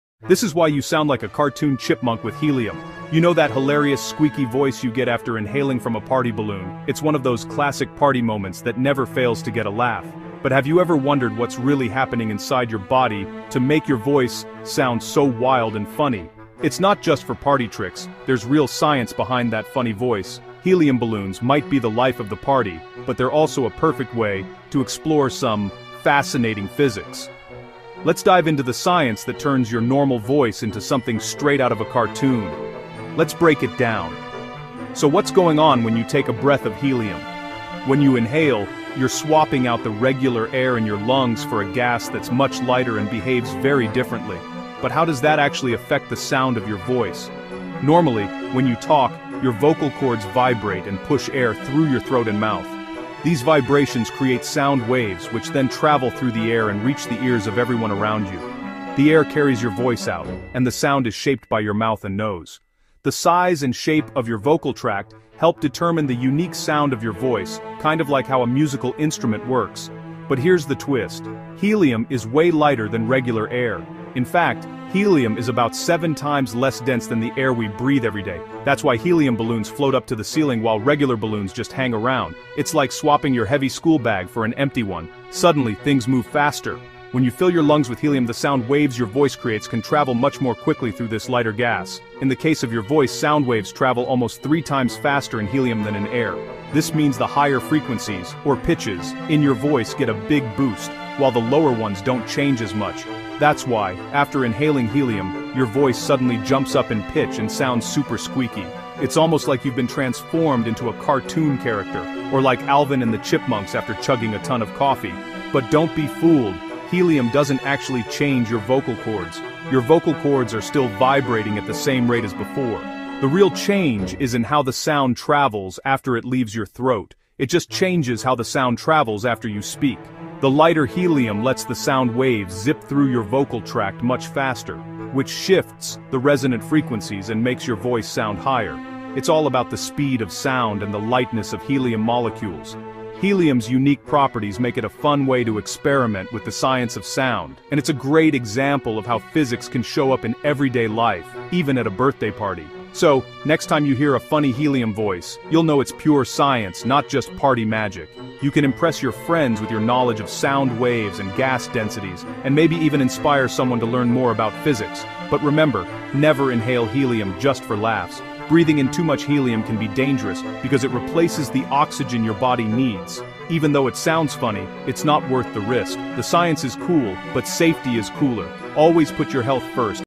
Why helium changes your voice
Ever wondered why a lungful of helium turns your voice into a hilarious, squeaky cartoon sound? It's not the pitch of your vocal cords changing, it's pure physics! This video dives into the fascinating science behind the classic party trick. When you inhale helium, you're filling your vocal tract with a gas much less dense than the normal air (nitrogen and oxygen). Since sound waves travel nearly three times faster in helium, the resonant frequencies—or formants—of your vocal tract are drastically increased. Your vocal cords still vibrate at the same rate, but your mouth and throat now amplify the higher-frequency harmonics, giving your voice that distinct, high-pitched timbre that sounds so funny. We break down the mechanics of the human voice, explain the science of resonance, and even show you what happens with a much heavier gas that has the opposite, deep-voice effect! Learn the truth behind the squeak, and remember to use helium responsibly—a little bit of science is always a good time!